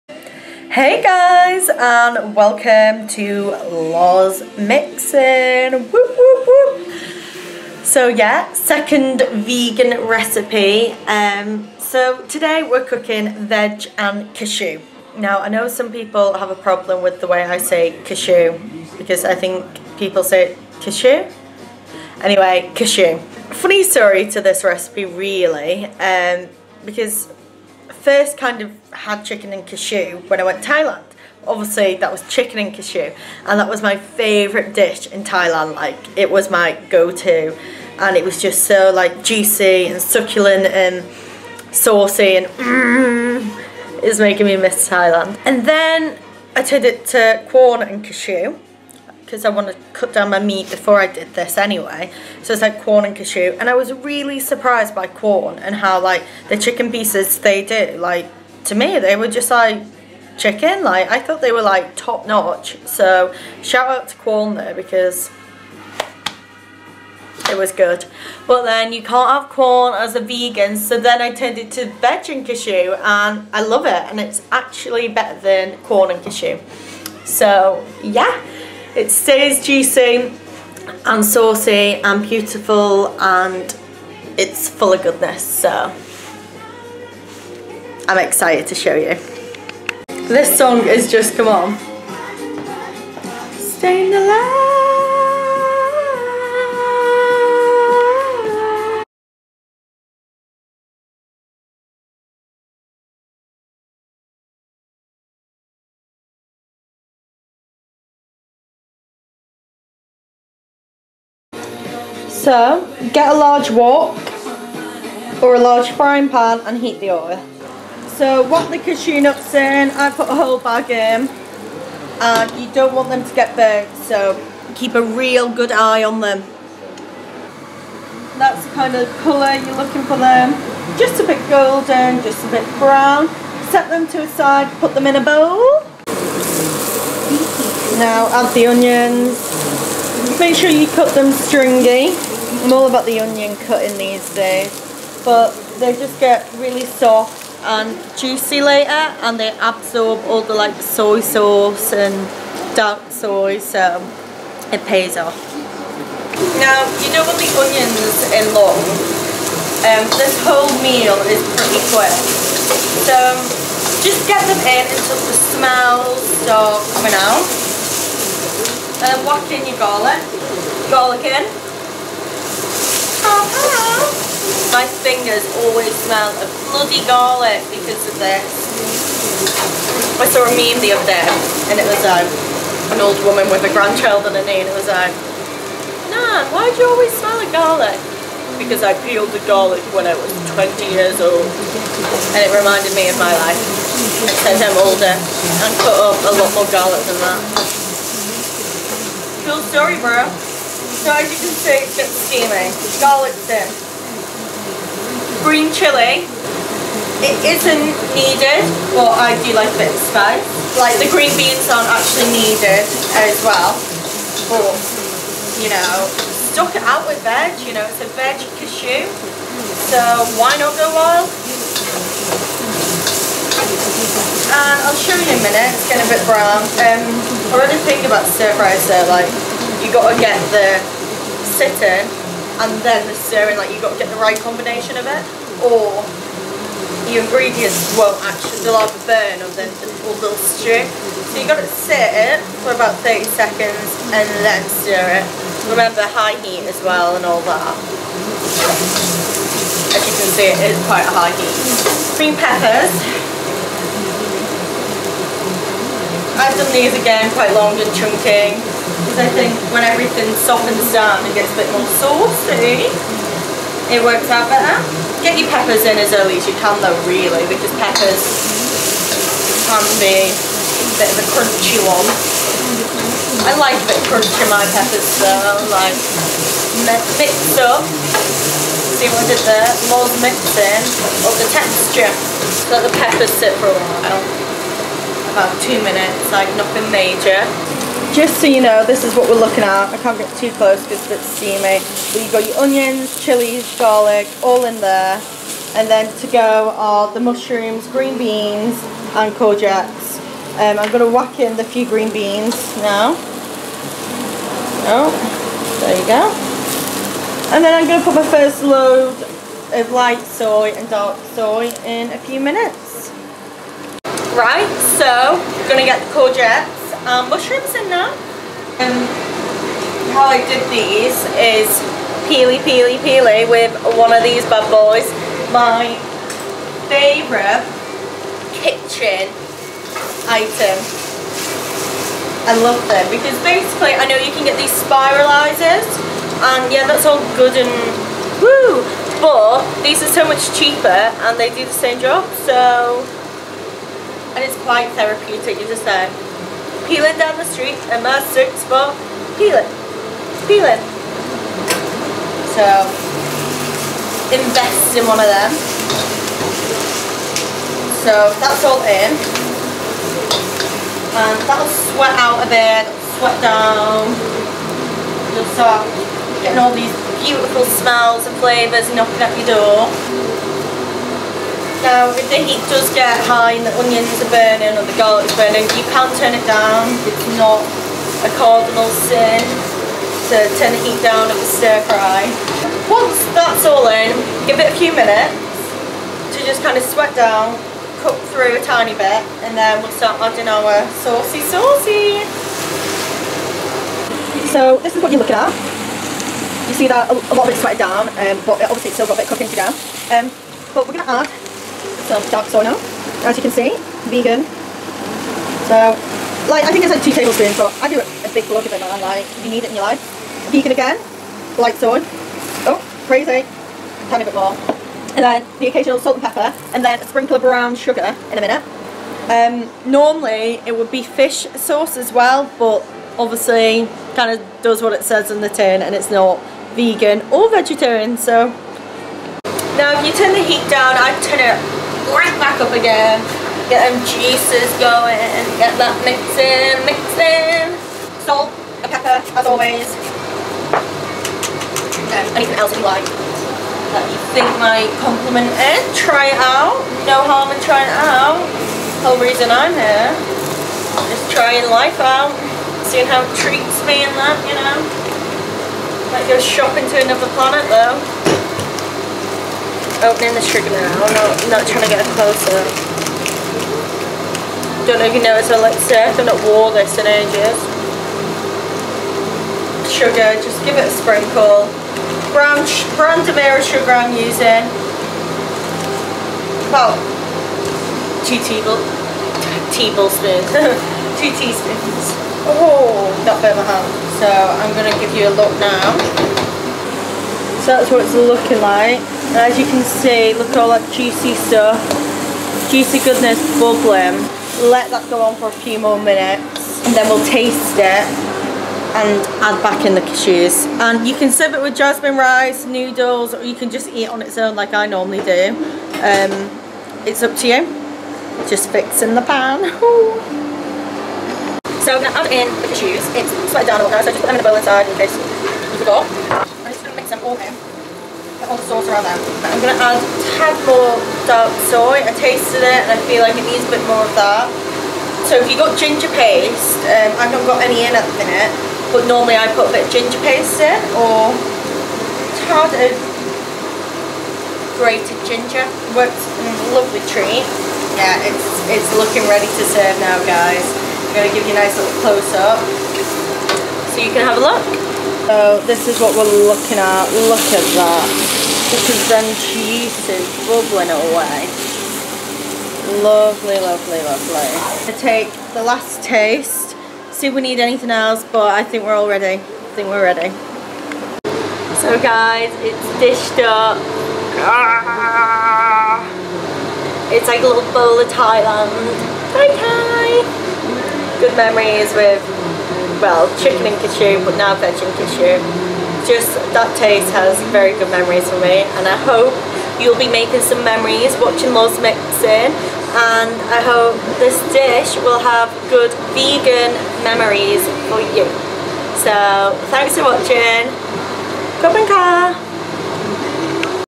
Loz Mixin's - Veg and Cashew - Vegan special
This is my go to dish, even months after not being vegan I still cook this delicious dish.
You can throw any left over veg in, mix it up yourself.
Here are mine go to ingredients...

A bag of cashew nuts
Chestnut mushrooms
Corgette
Green pepper
Onion
Spinach (opt)
Green chilli (opt)
Soy sauce
Dark soy
Ginger (opt)
Brown sugar
Corriander -fresh (opt)

and of course salt and pepper

Yummayyyyyy

Enjoy. Share and keep loving food and life !

Sorry it's been a while coming but theres only me that cooks,edits and acts for a life !

Cook. Sing. Dance. Enjoy.

@lozbitchin on insta
@loz_bradshaw on twitter